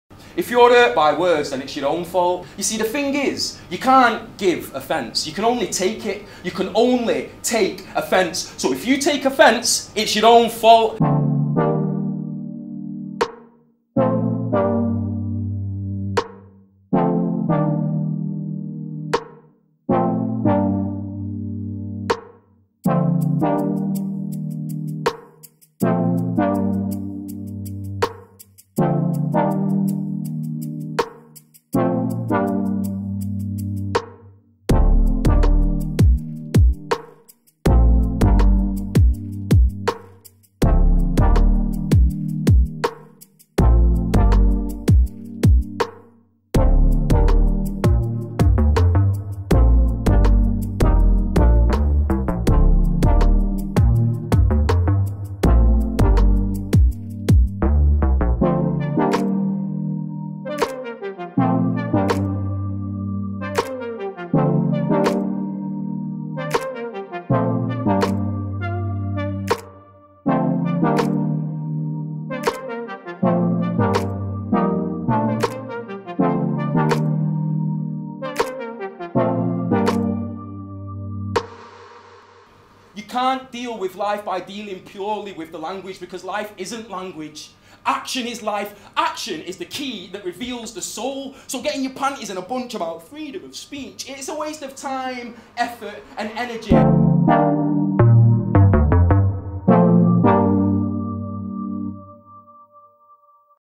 Slingshot Event 2016
A cross-over event combining four pieces of controversial writing with food, drink, politics and theatre.